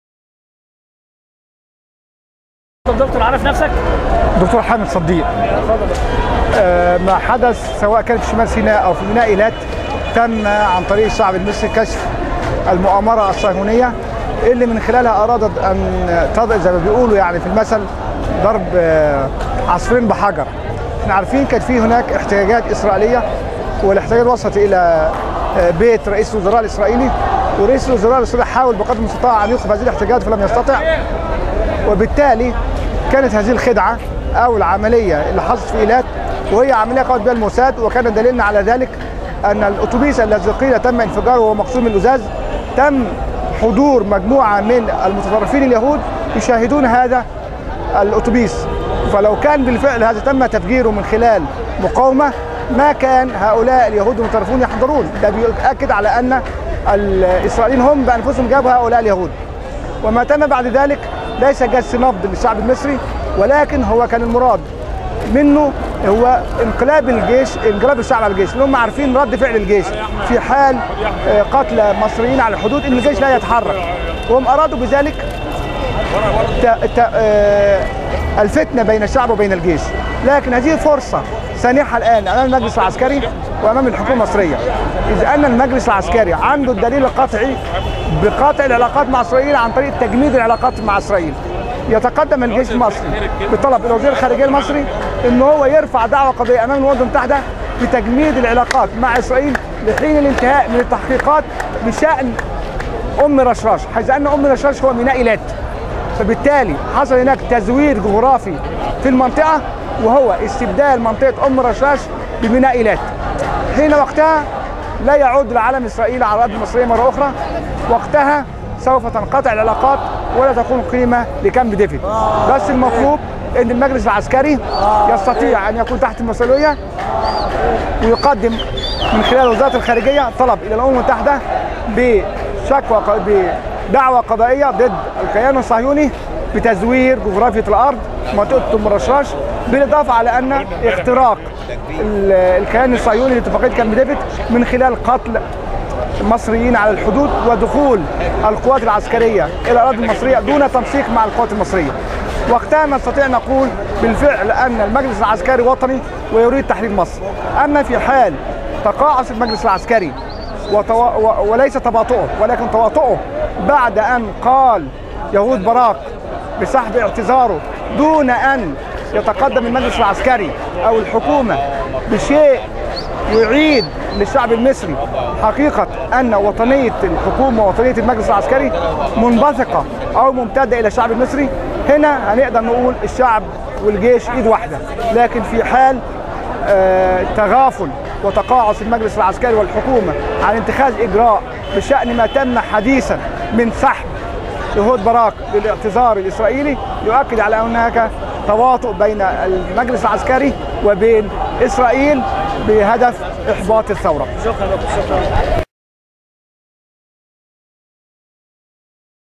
الرجل الذى قال فى محاكمة مبارك أنه قد مات من 2004
الرجل الذى قال فى محاكمة مبارك أن مبارك قد مات من 2004 فى لقاء مهم له من أمام السفارة الإسرائيلية